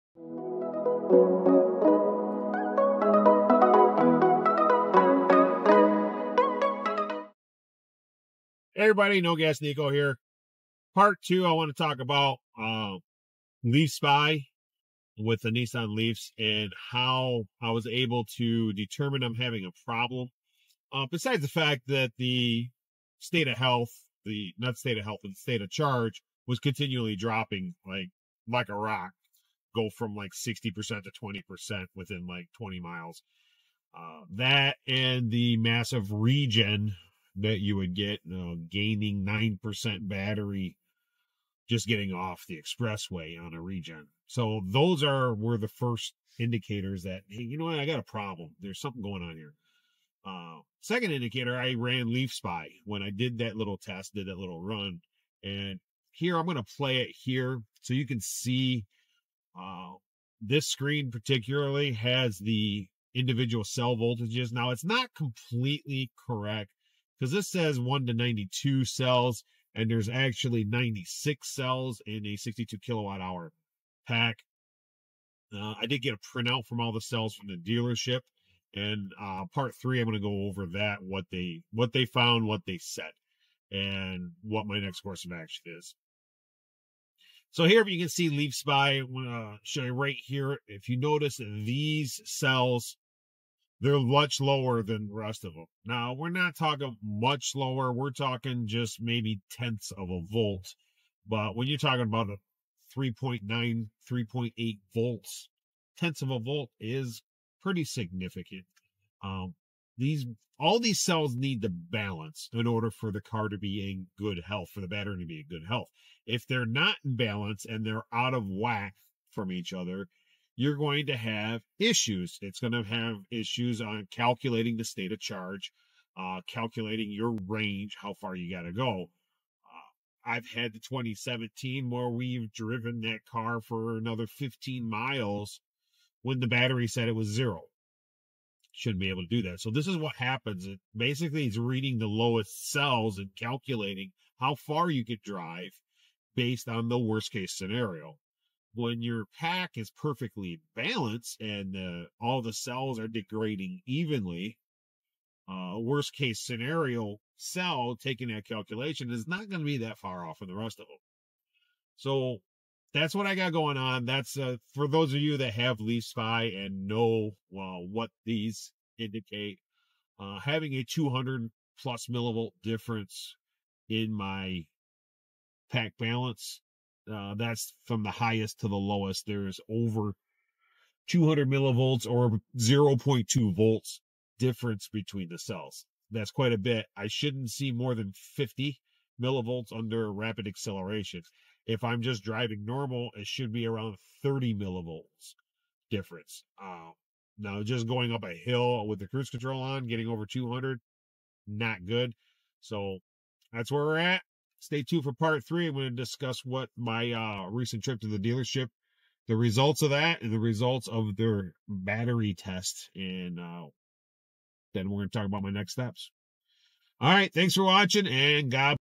62 kWh Battery Failure Nissan Leaf e+ - Part 2
LeLink BT OBD-II Device that works with LeafSpy

Part 2 - Using LeafSpy to determine what is going on with my battery pack.

Drop me a comment if you have one of these and have similar experiences.